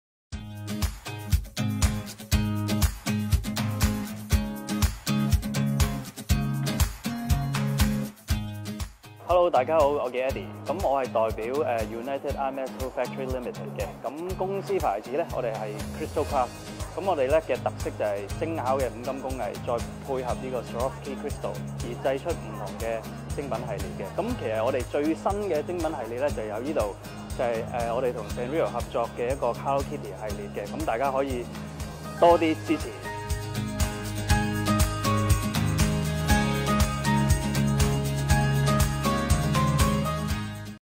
Crystocraft featured on HKTDC Exhibition Channel
Crystocraft exhibited in Hong Kong Gifts & Premium Show 2017 April 27 - 30 Hall of Fine Design. New collection included crossover with Hello Kitty and was featured on HKTDC Exhibition Channel.